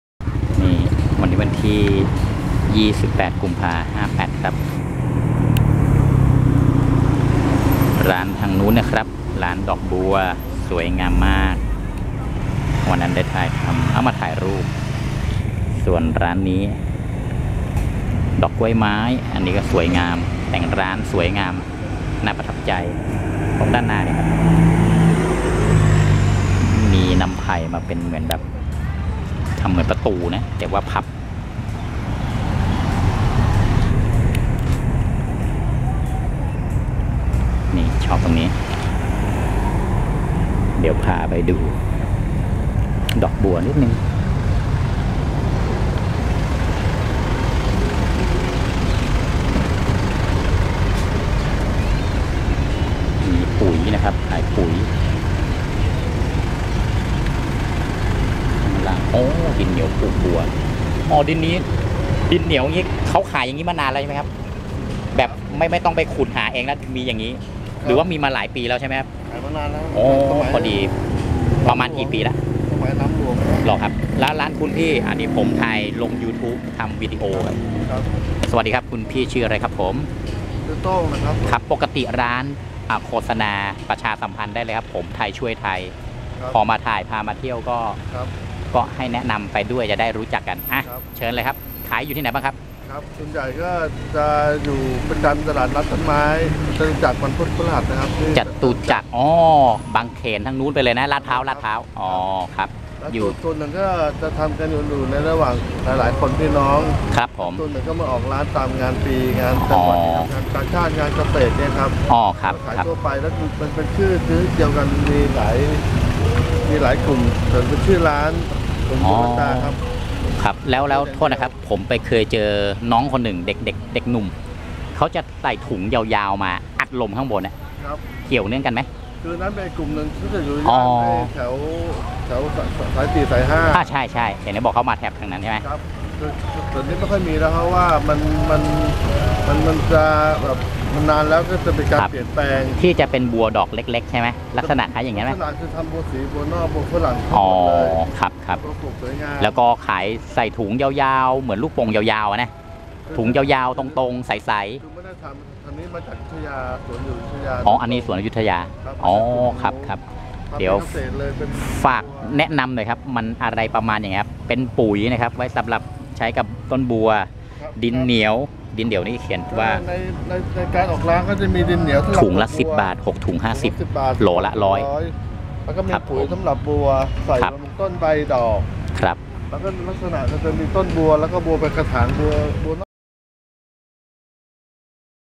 ดินเหนียวปลูกบัวขายเป็นถุงๆ : Tulip ; Shop
ดินเหนียวปลูกบัวขายเป็นถุงๆ  : Tulip ; Shop / เดินเที่ยวแวะทักทายร้านบัวครับ ไปดูว่าถ้าอยากซื้อไปเจอได้ที่ไหนครับ